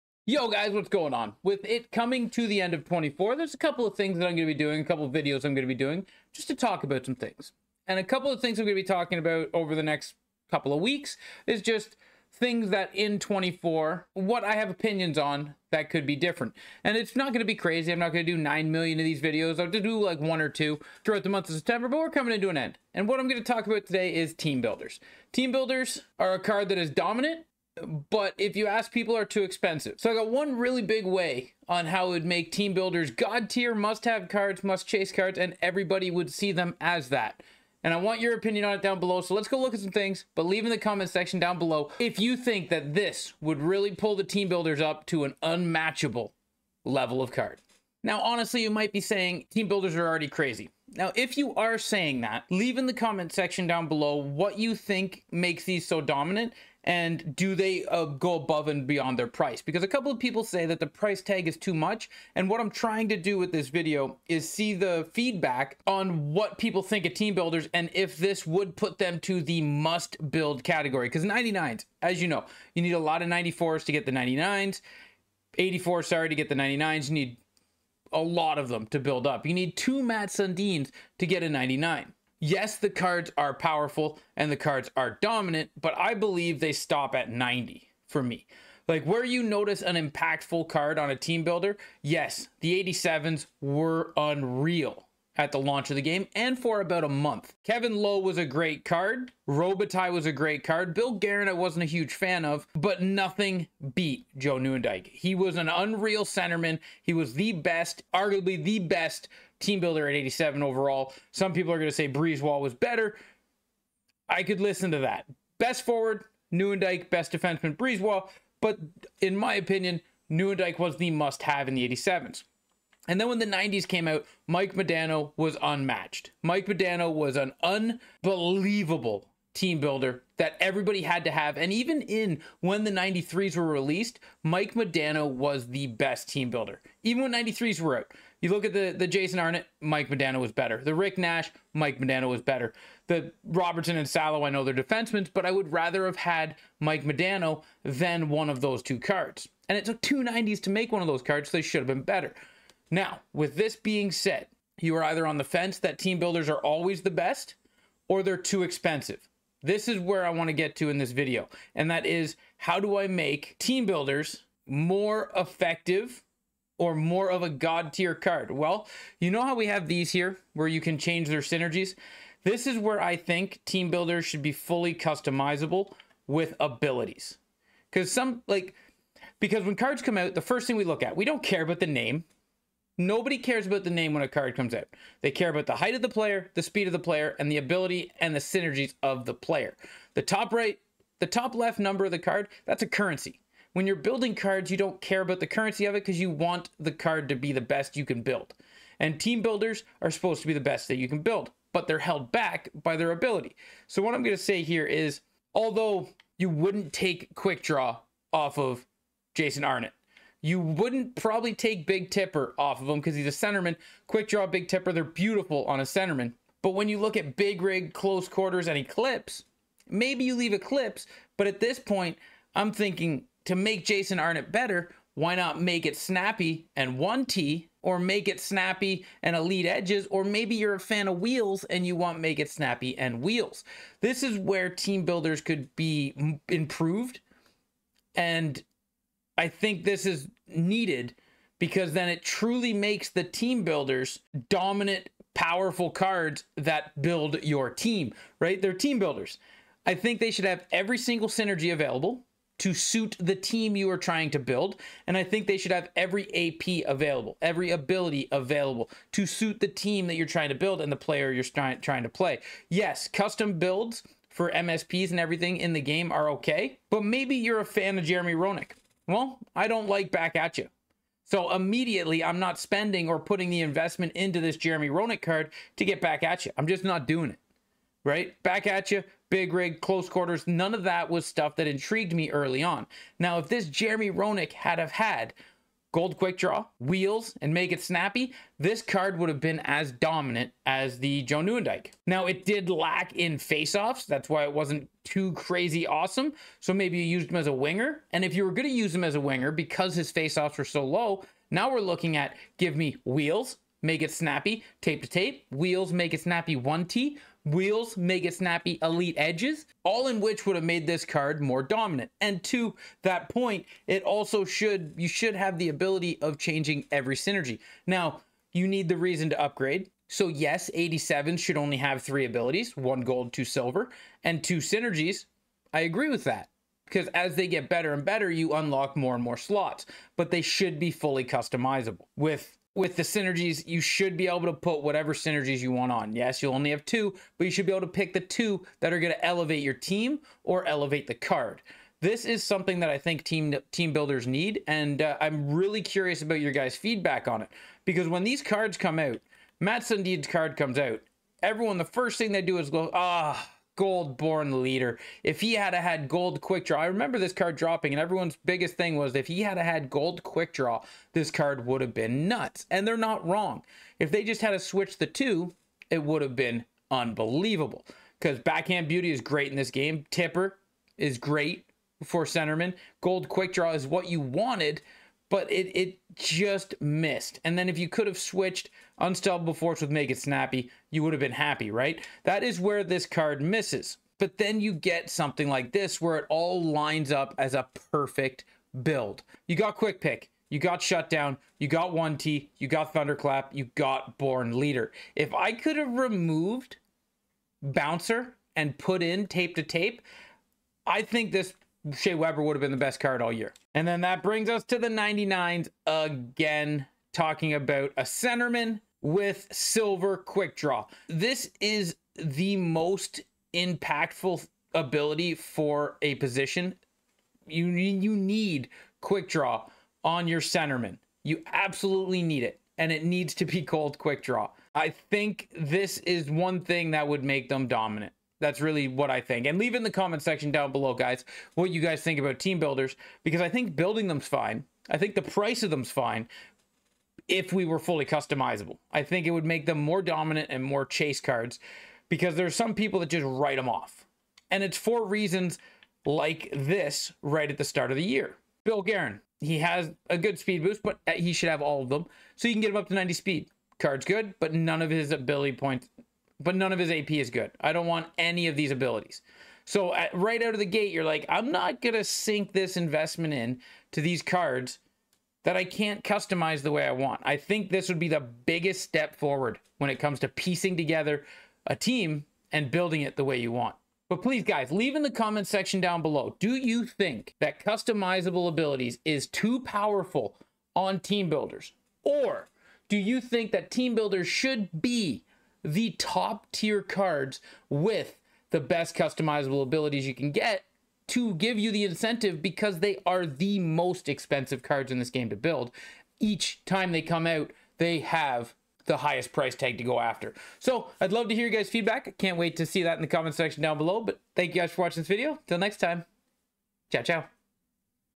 How to make TEAM BUILDERS Better and worth the cost
This would make team builders must build sets and best cards in the game